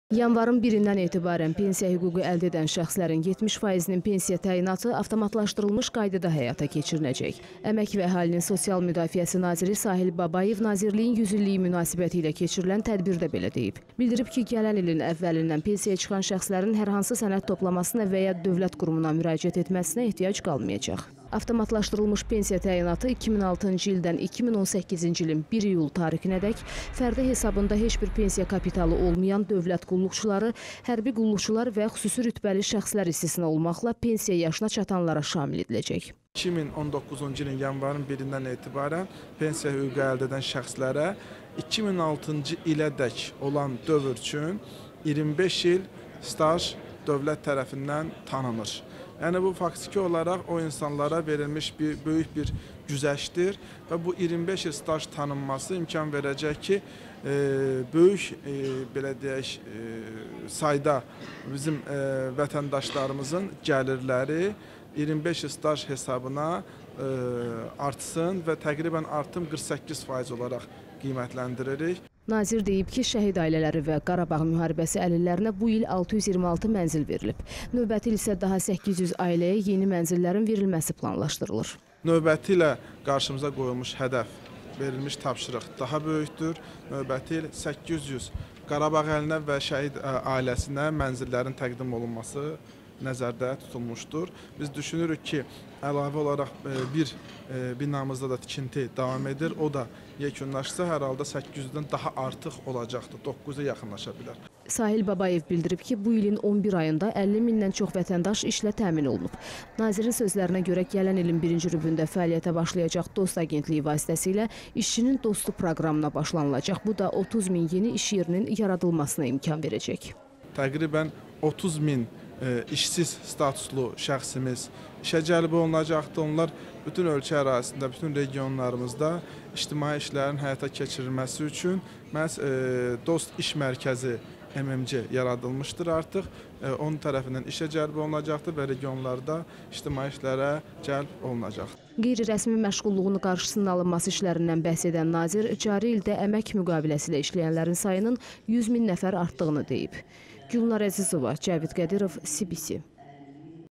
“İŞÇİNİN DOSTU” PROQRAMINA BAŞLANILACAQ
Nazir deyib ki, şəhid ailələri və Qarabağ müharibəsi əlillərinə bu il 626 mənzil verilib. Növbəti il isə daha 800 ailəyə yeni mənzillərin verilməsi planlaşdırılır. Sahil Babayev bildirib ki, bu ilin 11 ayında 50 mindən çox vətəndaş işlə təmin olunub. Nazirin sözlərinə görə, gələn ilin birinci rübündə fəlaiyyətə başlayacaq DOST agentliyi vasitəsilə “İşçinin dostu” proqramına başlanılacaq. Bu da 30 min yeni iş yerinin yaradılmasına imkan verəcək.

Qeyri-rəsmi məşğulluğun qarşısının alınması işlərindən bəhs edən nazir cari ildə əmək müqaviləsi ilə işləyənlərin sayının 100 min nəfər artdığını deyib.